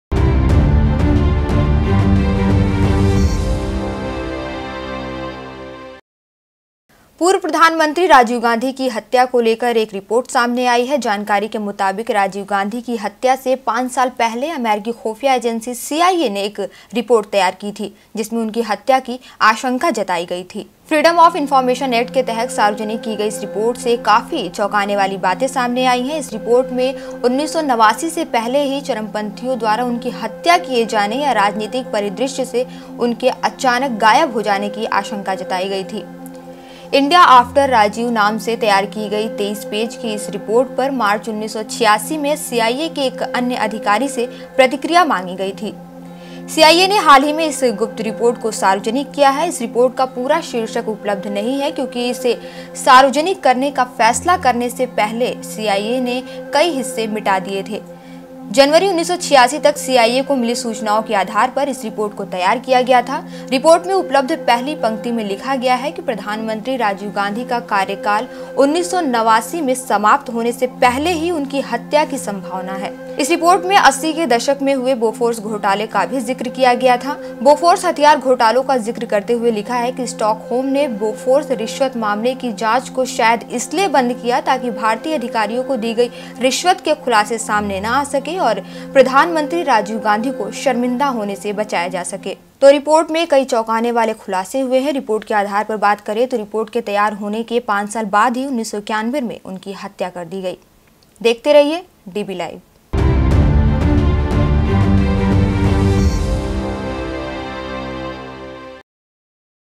DB LIVE | 31.1.17 | CIA had detailed brief on Rajiv Gandhi assassination
पूर्व प्रधानमंत्री राजीव गांधी की हत्या को लेकर एक बड़ा खुलासा सामने आया है। जानकारी के मुताबिक राजीव गांधी की हत्या से 5 साल पहले अमेरिकी खुफिया एजेंसी CIA ने एक रिपोर्ट तैयार की थी, जिसमें उनकी हत्या की आशंका जताई गई थी। फ्रीडम ऑफ इन्फॉर्मेशन ऐक्ट के तहत सार्वजनिक की गई इस रिपोर्ट से काफी चौकाने वाली बातें सामने आई हैं। इस रिपोर्ट में 1989 से पहले ही चरमपंथियों द्वारा उनकी हत्या किए जाने या राजनीतिक परिदृश्य से उनके अचानक गायब हो जाने की आशंका जताई गई थी। 'इंडिया आफ्टर राजीव. ' नाम से तैयार की गई 23 पेज की रिपोर्ट पर मार्च 1986 में सीआईए के अन्य अधिकारियों से प्रतिक्रिया मांगी गई थी। सीआईए ने हाल ही में इस गुप्त रिपोर्ट को सार्वजनिक किया है। इस रिपोर्ट का पूरा शीर्षक उपलब्ध नहीं है, क्योंकि इसे सार्वजनिक करने का फैसला करने से पहले सीआईए ने कई हिस्से मिटा दिए थे। जनवरी 1986 तक सीआईए को मिली सूचनाओं के आधार पर इस रिपोर्ट को तैयार किया गया था। रिपोर्ट में उपलब्ध पहली पंक्ति में लिखा गया है, 'प्रधानमंत्री राजीव गांधी का कार्यकाल 1989 में समाप्त होने से पहले ही उनकी हत्या की संभावना है।' इस रिपोर्ट में 80 के दशक में हुए बोफोर्स घोटाले का भी जिक्र किया गया था। बोफोर्स हथियार घोटालों का ज़िक्र करते हुए लिखा है कि स्टॉकहोम ने बोफोर्स रिश्वत मामले की जांच को शायद इसलिए बंद किया, ताकि भारतीय अधिकारियों को दी गई रिश्वत के खुलासे सामने न आ सकें और प्रधानमंत्री राजीव गांधी को शर्मिंदा होने से बचाया जा सके।तो रिपोर्ट में कई चौंकाने वाले खुलासे हुए हैं। रिपोर्ट के आधार पर बात करें तो रिपोर्ट के तैयार होने के पांच साल बाद ही 1991 में उनकी हत्या कर दी गई थी।